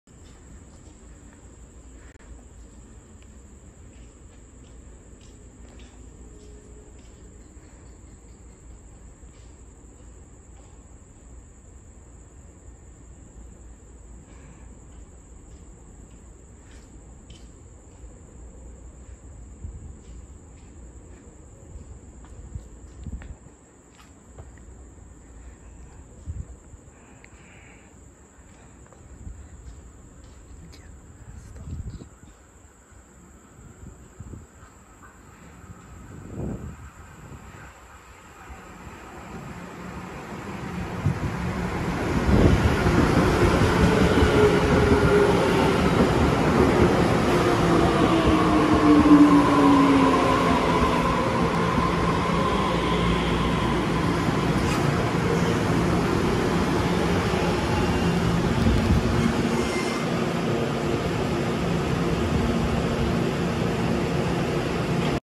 285次太魯閣自強號往彰化進入清水站
拍攝時間：2022/7/22 22:40
車號：(彰化)TED1009+TED1010(台北.花蓮)
沿途停靠：花蓮、羅東、宜蘭、七堵、松山、台北、板橋、鶯歌、桃園、中壢、新竹、竹南、後龍、通霄、苑裡、大甲、清水、沙鹿、彰化。